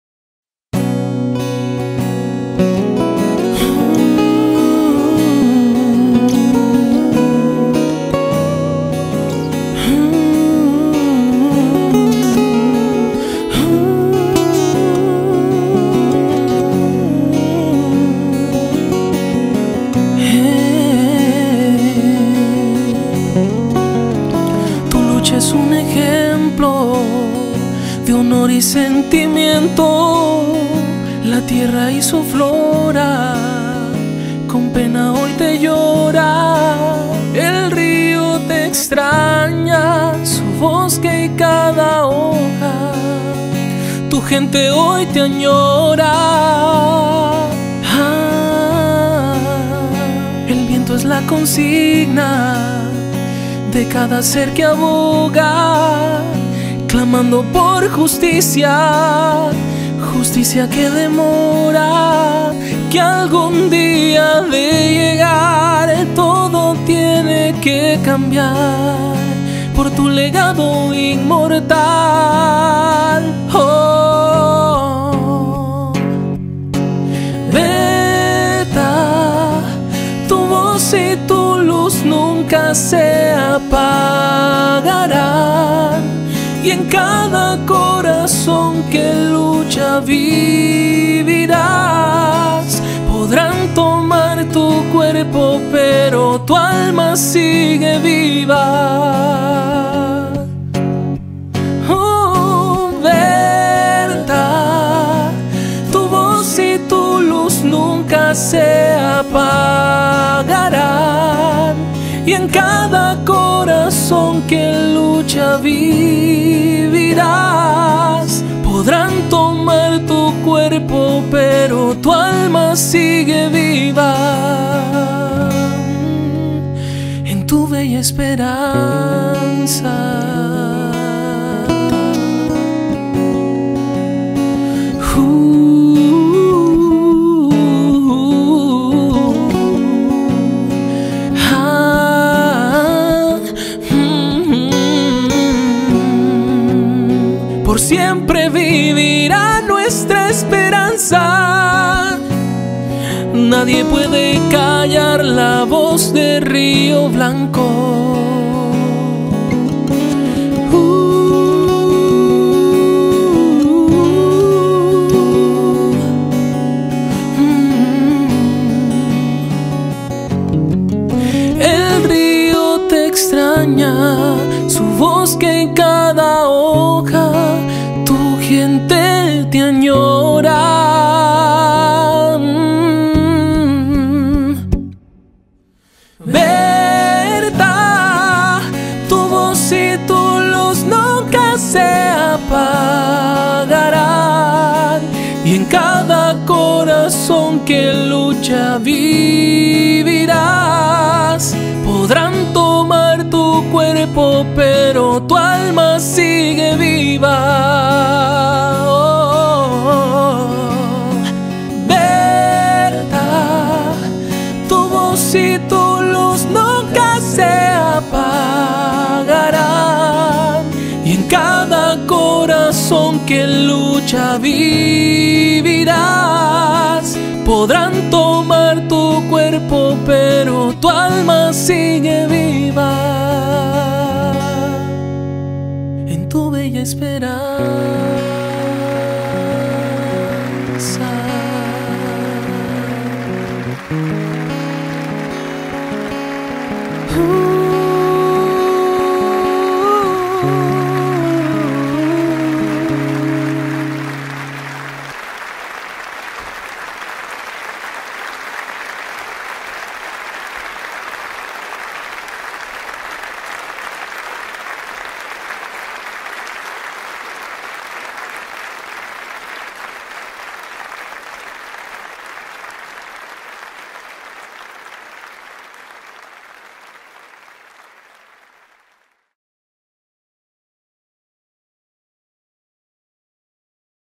Video Homenaje Berta Caceres
Este vídeo es un pequeño homenaje a Berta Cáceres (Q.E.P.D.), una mujer a quien Honduras recordará siempre por su lucha en pro de la Comunidad Indígena Lenca.
Tema musical interpretado por el cantante y compositor sampedrano Liam Rivera Liam Musica .
Edición de Gerson Valladares para RTV Honduras